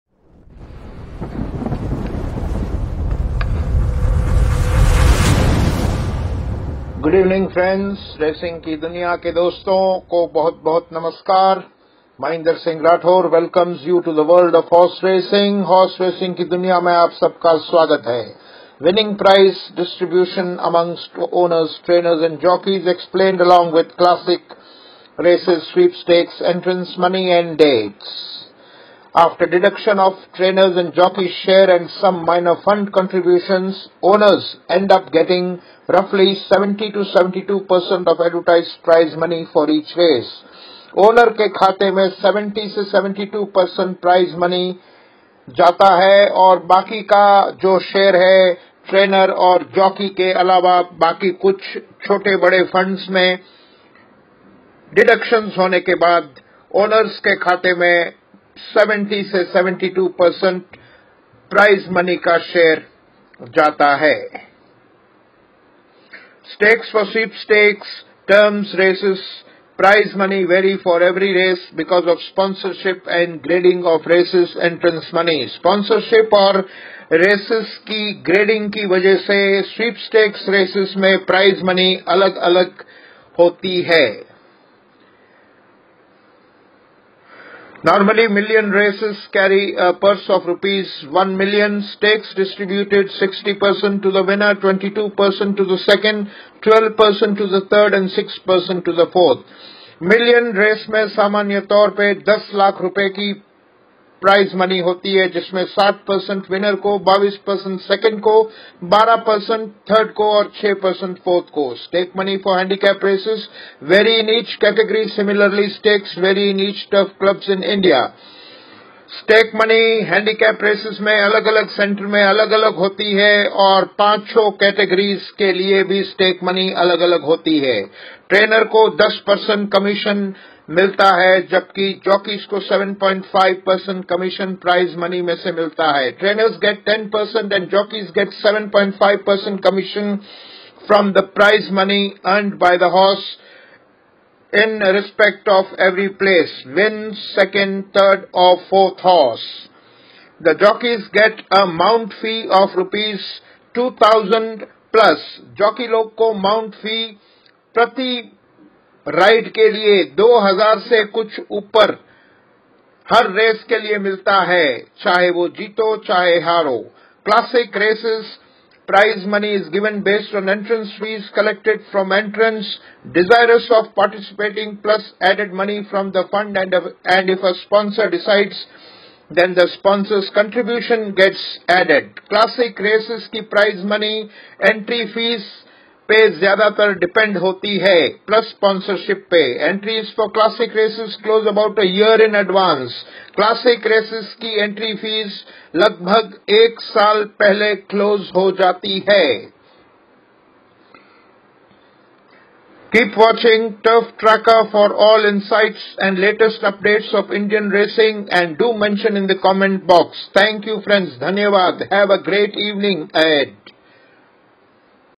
INTERESTING RACING FACTS EXPLAINED
I explain the Trainers, Jockeys and Owners stakes and commission percentage earned. And also the Classic/sweepstakes races facts and figures